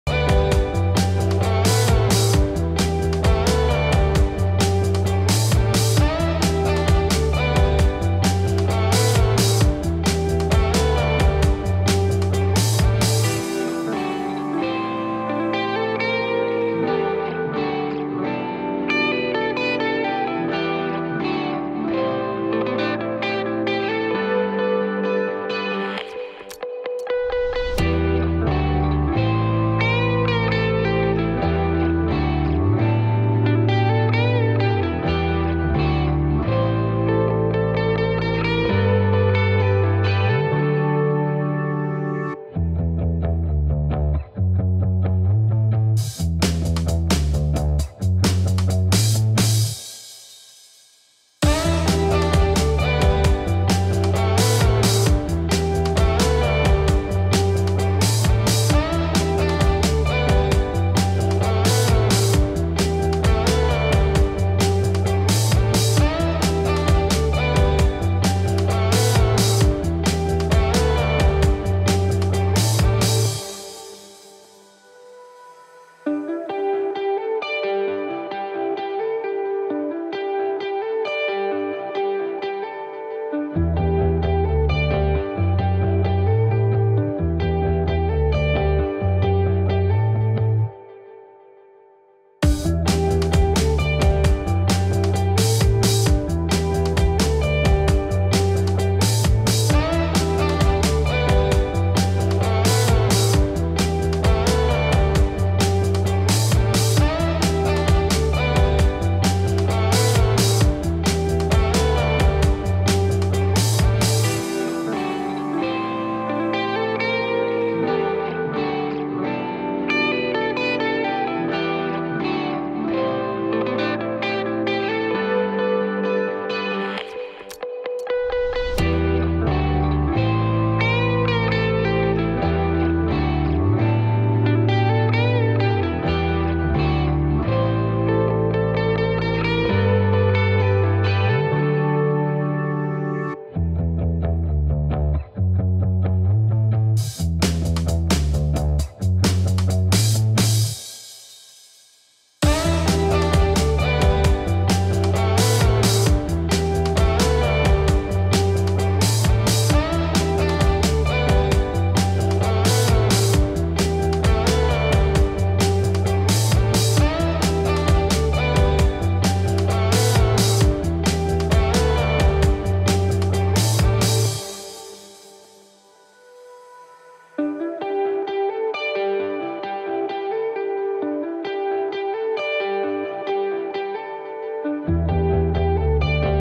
Chandra babu and Bala Krishna at Mahesh babu house | Vijaya Nirmala | Film jalsa
Chandrababu and Bala Krishna at Maheshbabu house | Vijaya Nirmala | Film jalsa